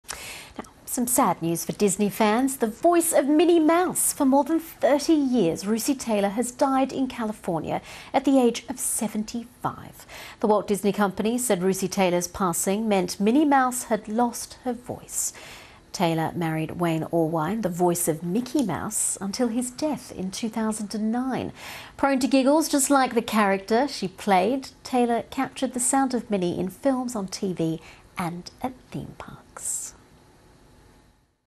Russi Taylor passes away (1944 - 2019) (USA) - BBC News - 28th July 2019
The voice of Disney's Minnie Mouse for 30 years, (and known for voice on 'The Simpsons' as well, along with many other TV and film voice overs), Russi Taylor has died at the age of 75 (about ten weeks after her birthday).